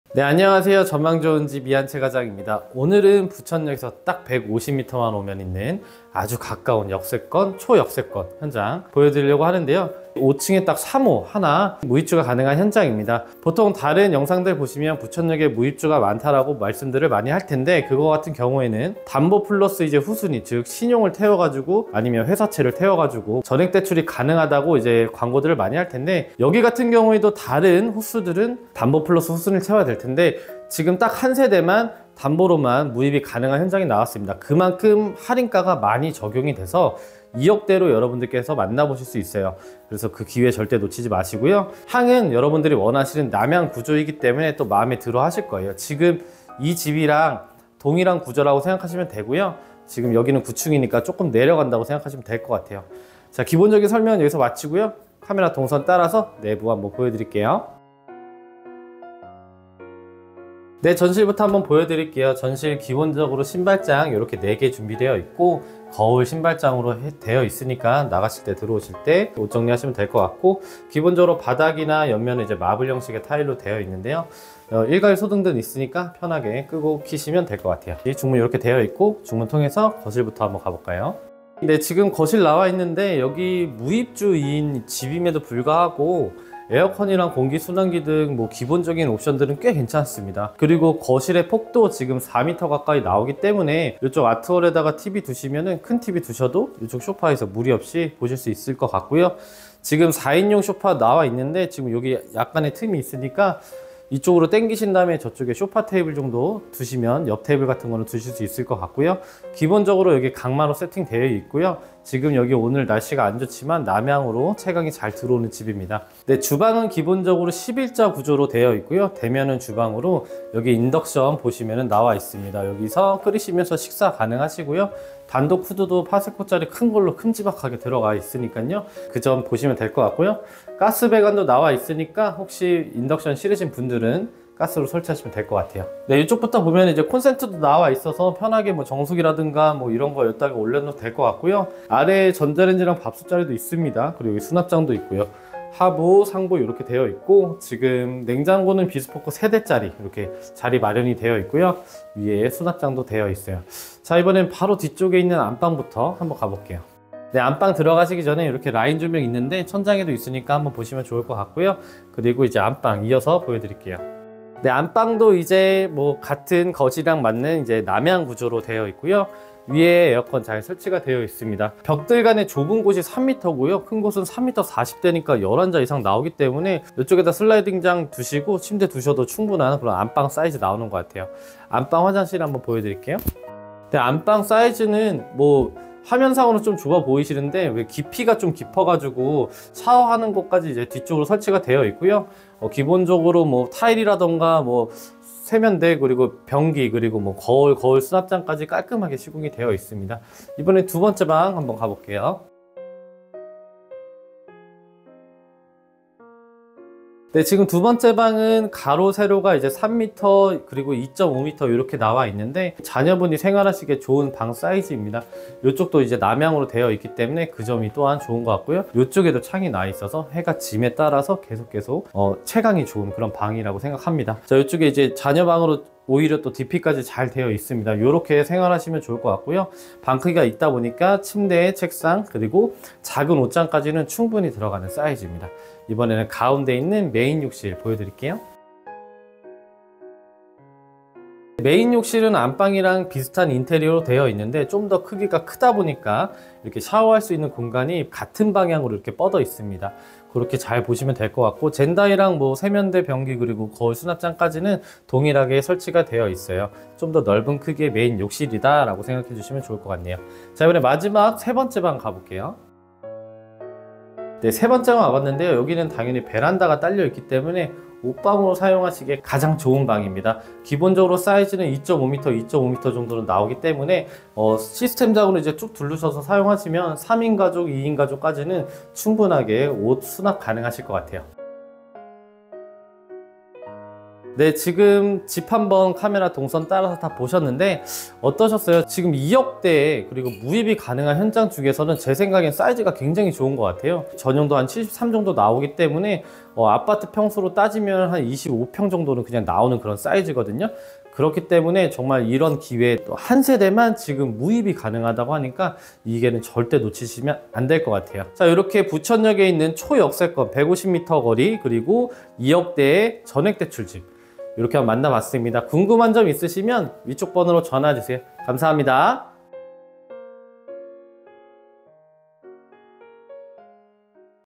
[부천신축아파트] 부천에 있는 2억대 중 제일 큰 집~! 거기다 담보 대출로만 전액대출?!😮부천역 150m로 3분이면 도착~!😁 (매물번호: 1775) [부천미분양아파트]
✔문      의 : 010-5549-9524

✔위      치 : 부천시 원미구 심곡동

✔교      통 :  1호선 부천역 도보 3분

✔분양가격 : (2억후반)
(🙏은행 대출 감정으로 기존 계약자분들이나 입주 세대에 영향을 미칠 수 있어 온라인상 정확한 금액 노출은 어려우니 유선문의 부탁드립니다!🙏)

✔필요 입주금 : 무입

✔구조 : 방 3, 욕실 2, 베란다 1

✔사이즈 : 27평형

✔옵션 : 시스템에어컨2개 공기순환기 1대, 인덕션1구(가스교체가능) 단독후드

✔주변환경 : 부천역 도보 3분
                 부천역 대형 상권 모두 도보 10분내
                 원미초 소명여고 도보 12분
                 가톨릭부천성모병원 도보 12분

✔특징 : 부천역 후순위(신용)대출 필요없는 진짜 전액대출현장
           부천역 1호선 직선거리 150m의 초역세권 현장
           부천에 몆개 안남은 2억대 현장
           2억대 현장 중 집 크기 가장 큰 현장